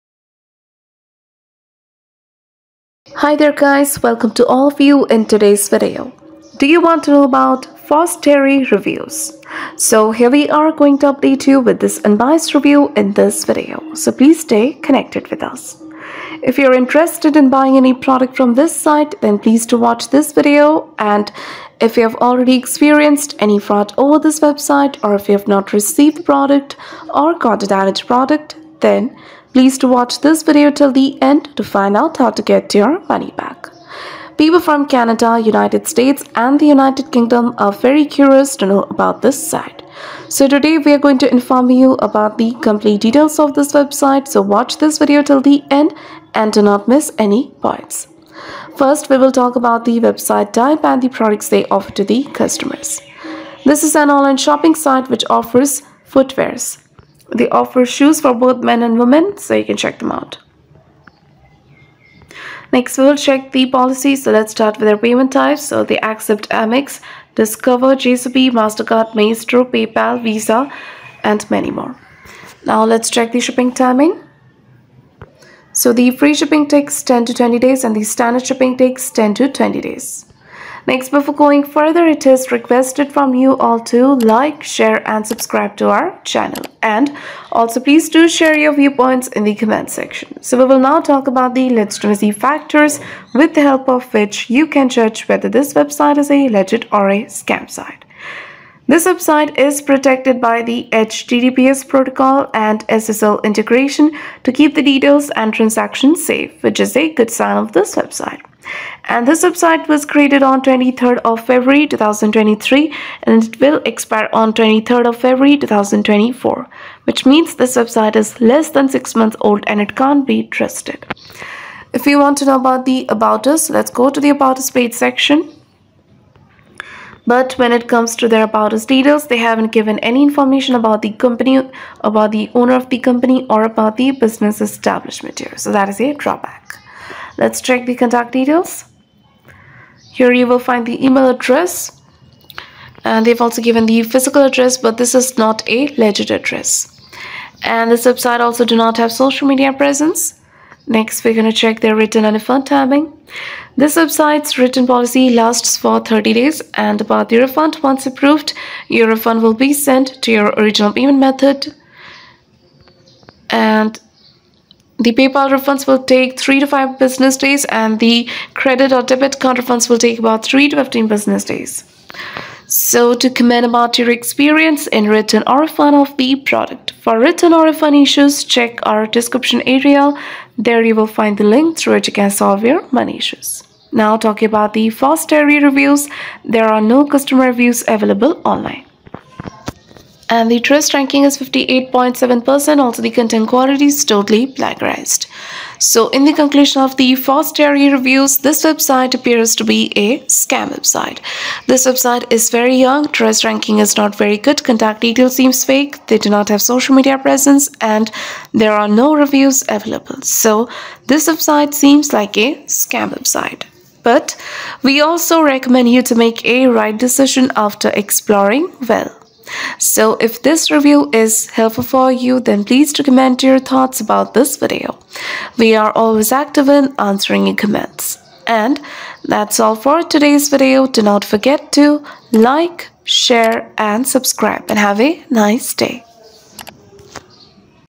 Fosterry Reviews (May 2023) - Is This A Legit Website? Find Out! | Scam Inspecter
Do you want to know about Fosterry Reviews?  Watch this video till end because in this video you will get to know about the legitimacy of the site.

Scam Inspecter